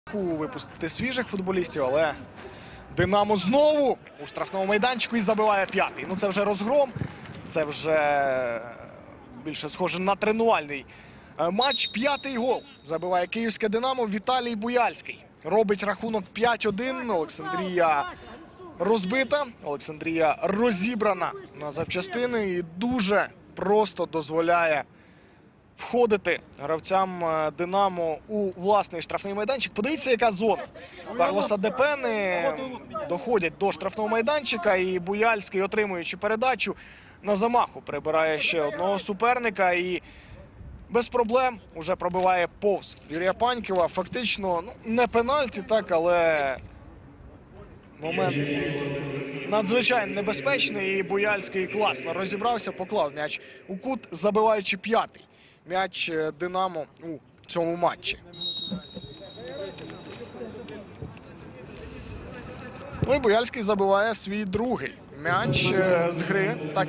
Динамо – Александрия - 5:1. Гол: Буяльский (83')
Чемпионат Украины 2019/2020. 25 тур. Динамо – Александрия - 5:1. Видео гола Буяльского на 83 минуте матча.